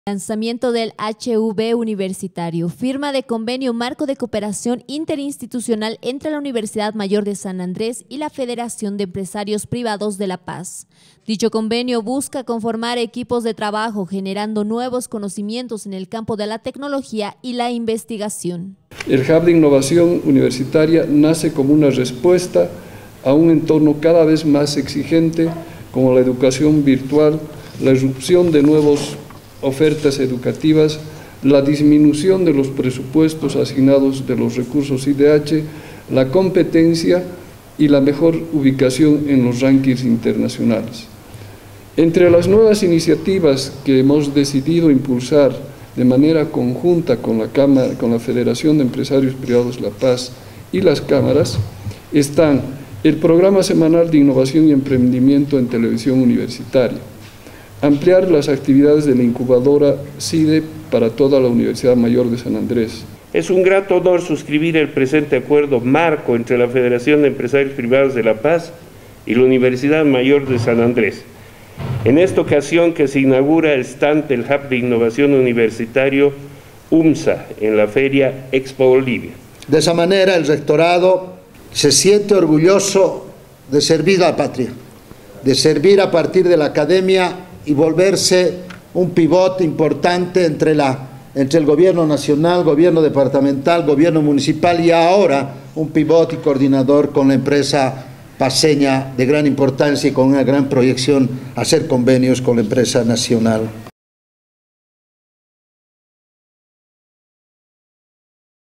Se firmó convenio entre la UMSA y la Federación de Empresarios
Se firmó convenio interinstitucioal entre la UMSA y la Federación de Empresarios Privados de La Paz.